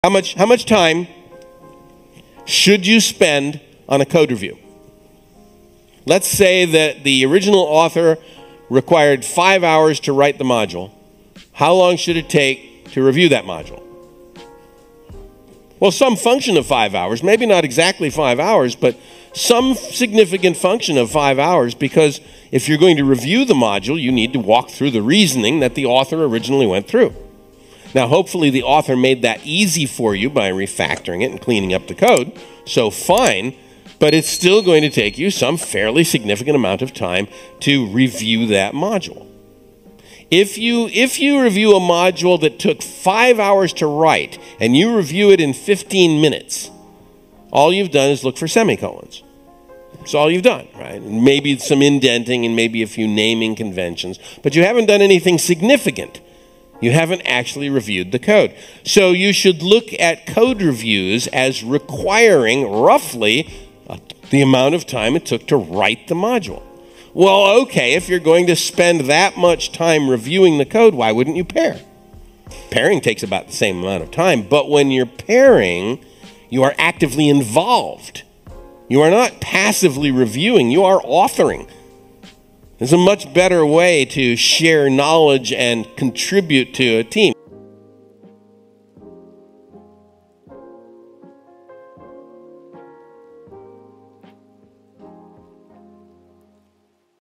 How much time should you spend on a code review? - Uncle Bob
In this video, Robert C. Martin (Uncle Bob) the author of the books Clean code and Clean Architecture speaks on how much time developers should dedicate towards code review in order to ensure code quality during the software development process.